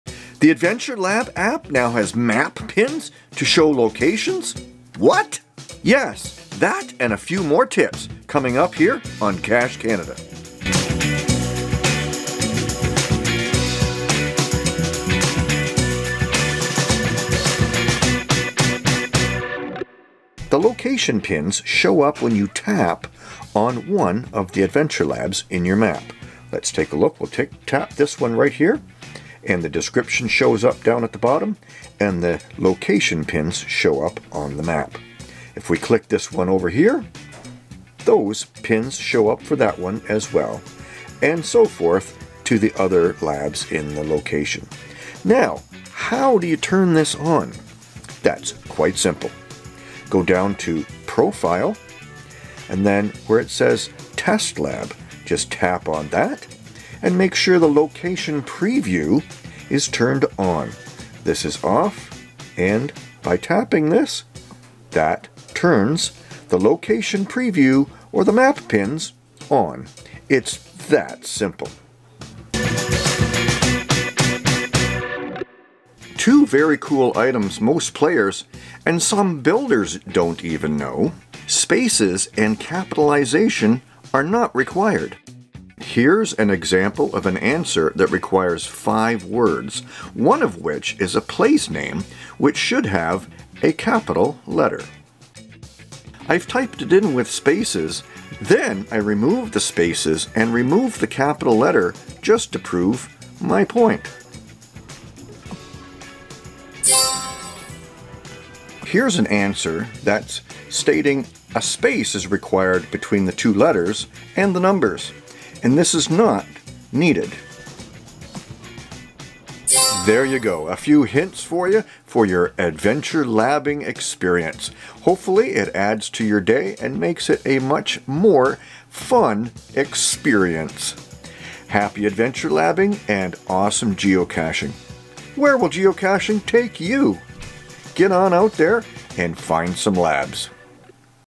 Adventure Lab Hints
The new location preview and a couple more basic hints for the Adventure Lab Geocaching app.